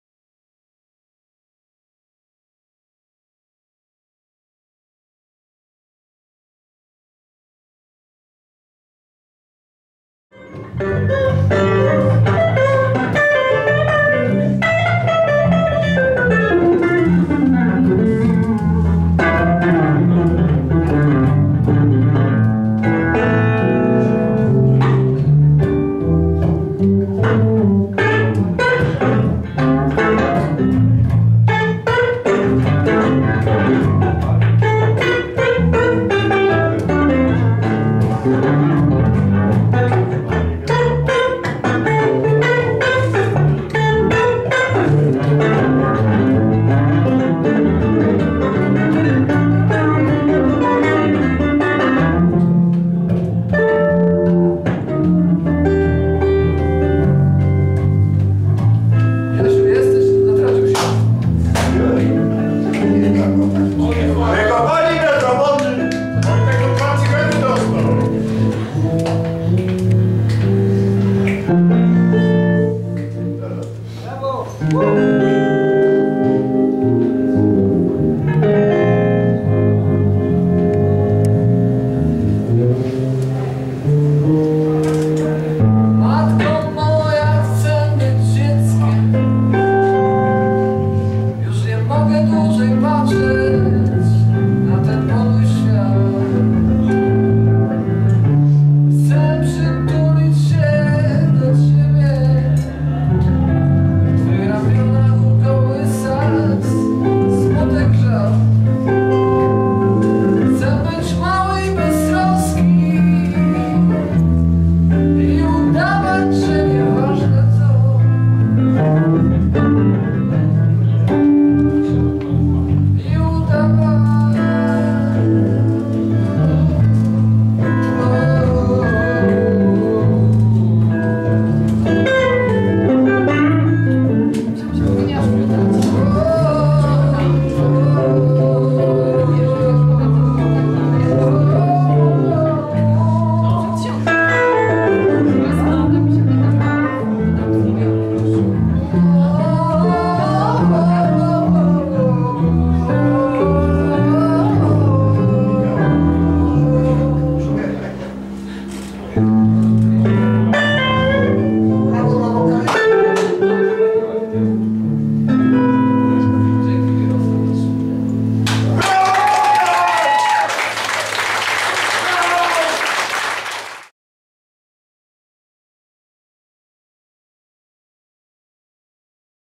BYŁ SOBIE BLUES 2014: Sam na sam z bluesem - Wojciech Klich, Mirosław Rzepa
Krótki występ Wojciecha Klicha i Mirosława Rzepy w formie jam session w ramach koncertu "Sam na sam z bluesem". Występ był częścią dziewiątej edycji festiwalu Był Sobie Blues (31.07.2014 r., Tarnów, Piwnice TCK).